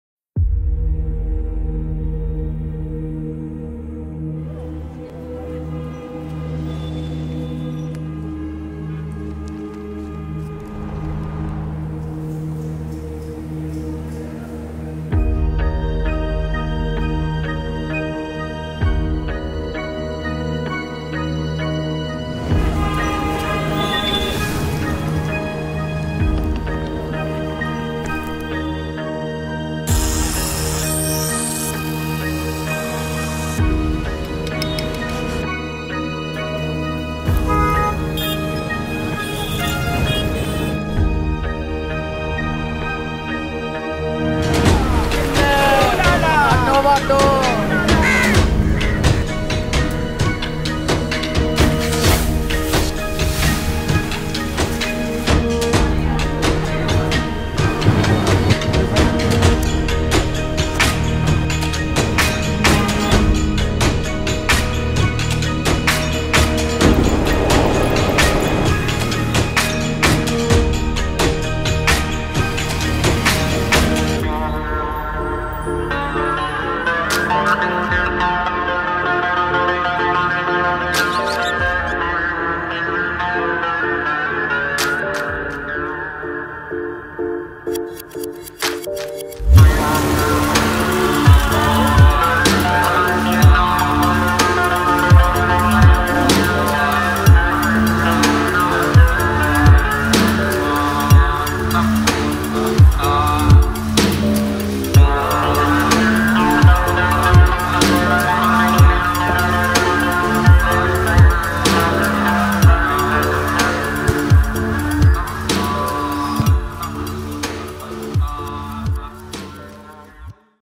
10 Days In India | Short Film
A cinematic video showcasing the diversity of India. A diverse country filled with love, passion and color.

In this video we explore Delhi, Jaipur, Agra, Ranthambore, and Kerala.